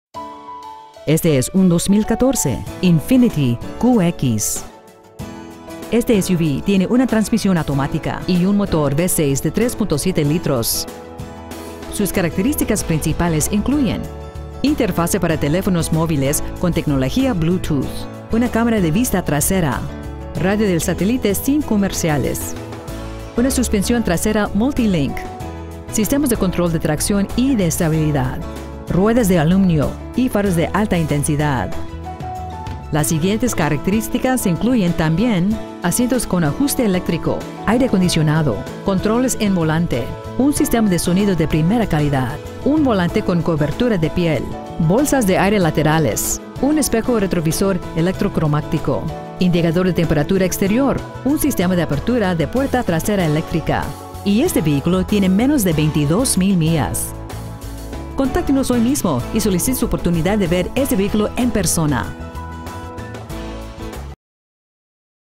Pre-Owned 2014 Infiniti QX70 Palm Beach FL
Haz clic aquí para ver el precio Hemos tenido el honor de servir a la zona de Lake Worth fl , le prometemos que su experiencia superará sus expectativas ! Year : 2014 Make : Infiniti Model : QX70 Engine : Motor con 6-Cil de 3.7L Trans . : Transmisión Automática Millaje : 21,412 Stock : 81216 Offleaseonly.com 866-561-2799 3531 Lake Worth Rd Lake Worth , fl 33461